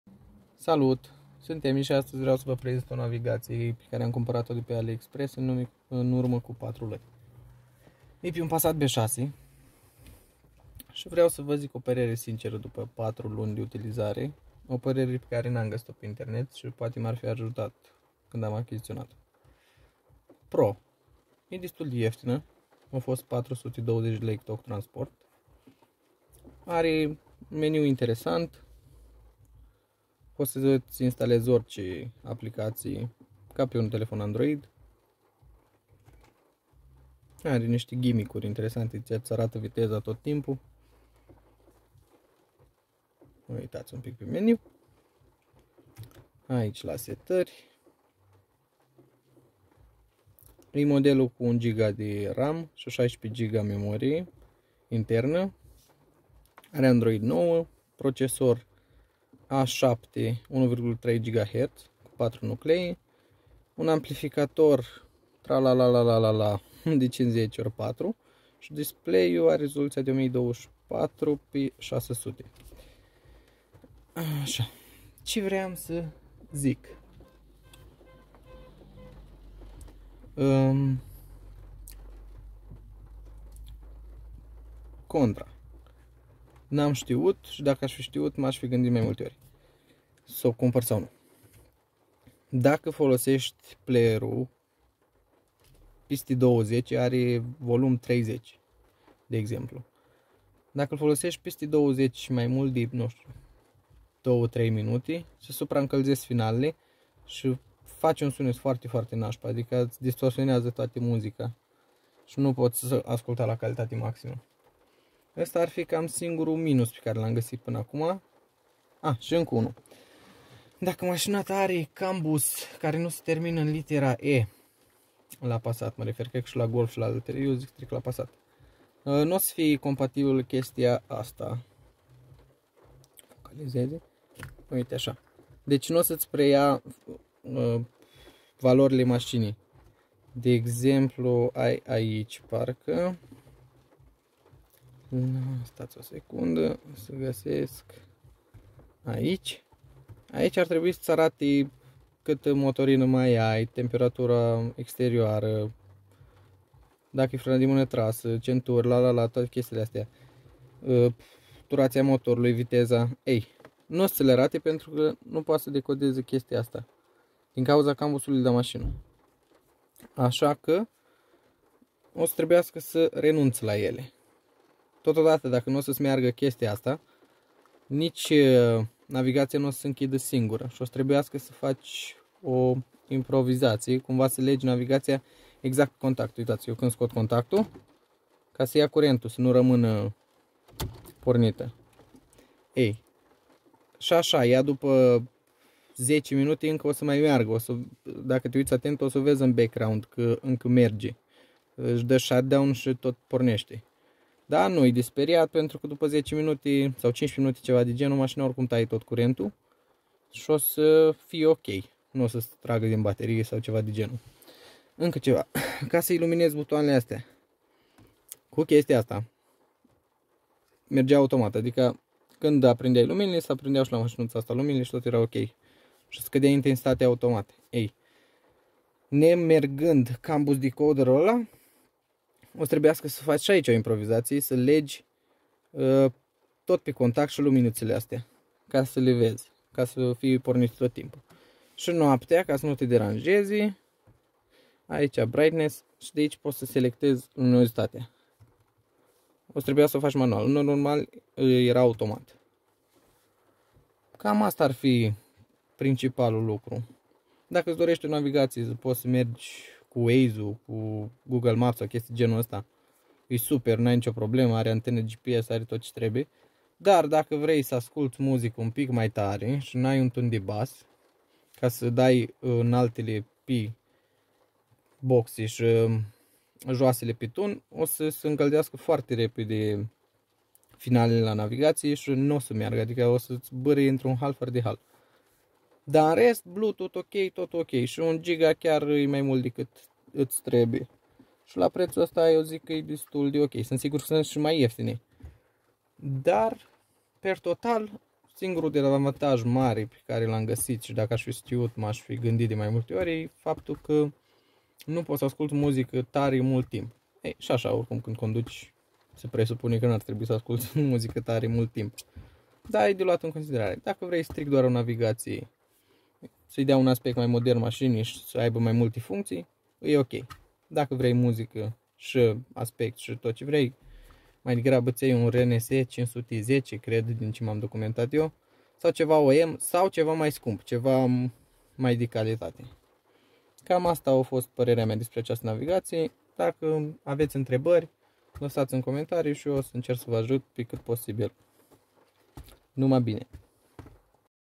Review Navigatie Android essgoo 9 pe Vw Passat b6
In acest vlog va prezint parerea mea sincera despre navigatia Essgoo 9" 1Gb ram si 16gb memorie interna.